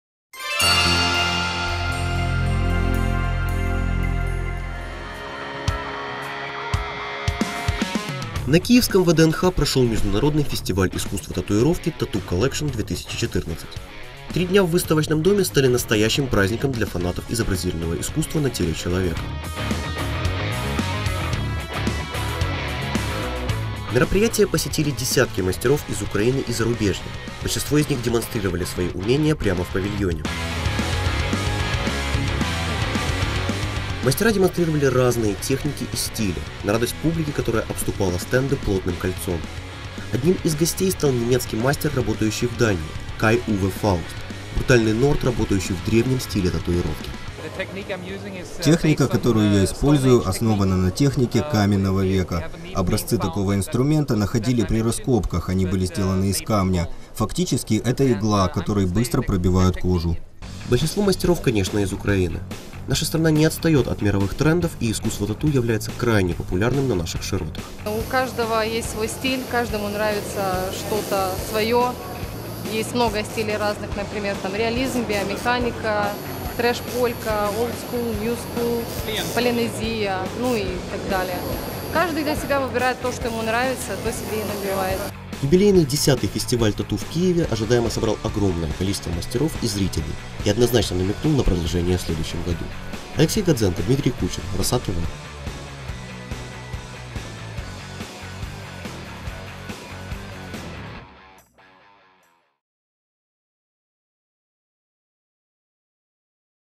Международный фестиваль татуировки «Tattoo Collection 2014»
Фестиваль «Tattoo Collection» - это ежегодный международный фестиваль-фурор в мире тату и пирсинга. Он настолько полюбился киевлянам и гостям столицы, что «Tattoo Collection » стал одним из самых ожидаемых событий этого года. Фестиваль объединяет множество людей из разных стран с одной целью – показать общественности, что тату – это не стереотипная « зэковская наколка», а  один из видов творчества на человеческом теле. Тату является выражением внутреннего мира и состояния человека, символом его мировоззрения.